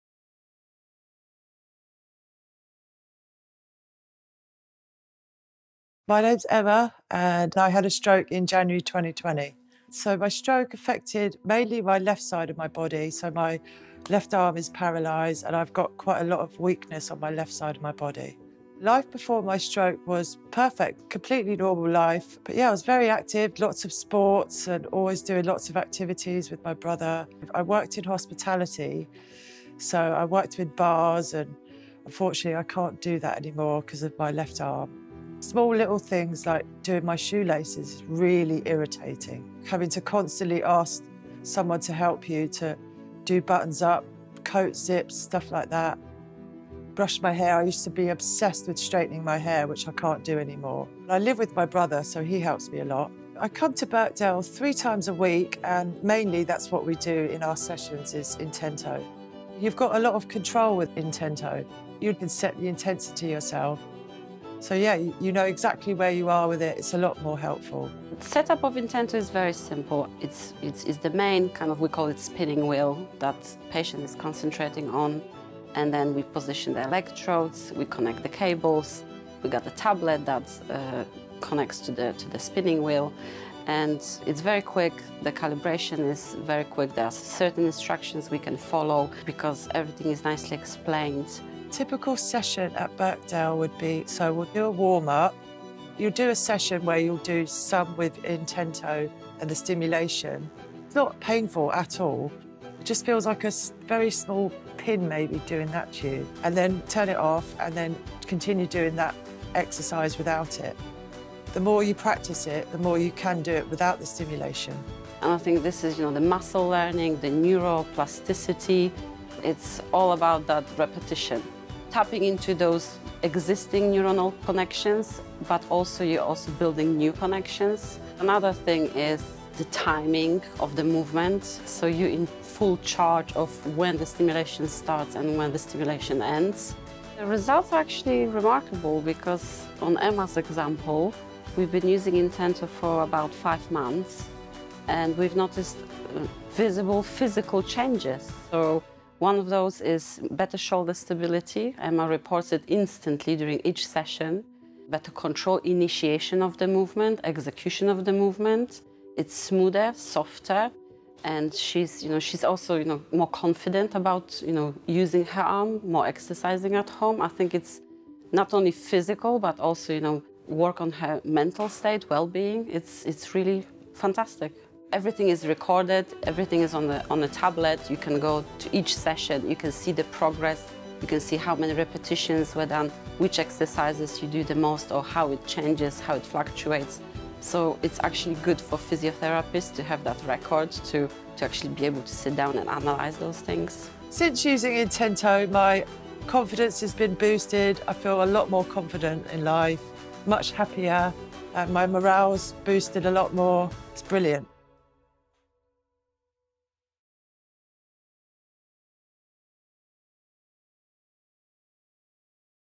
Intento Pro | Emma's Story
Emma suffered a stroke in 2020 that left her with impairment on the left side of her body. But a programme of self-modulated functional electrical stimulation with our Intento PRO treatment device is helping her to regain upper limb movement.